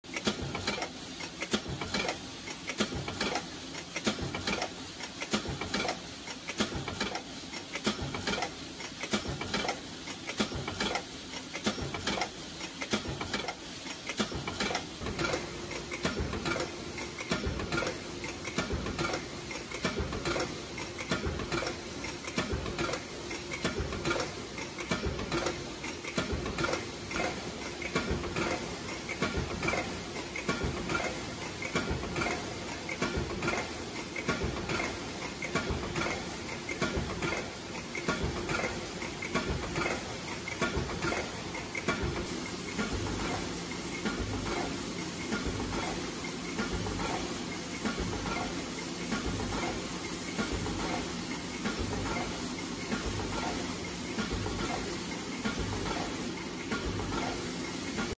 Code1635 Four Cavities Aluminum Foil Container Mold and making machine
Saudi Arabic market popular model 1635 aluminum foil container mold we have completed.

This is four cavities mold with high production capacity, working in CHOCTAEK's C1300 model machine. Its production speed is 60 strokes per minute, as you can see in the video, the machine and molds both working stable and very well.

And this production line fully automatically, punching automatic, automatic conveying, automatic counting and automatic stacking, also the foil scrap is atuomatically sucking and collecting.

For more specifications, please contact CHOCTAEK team.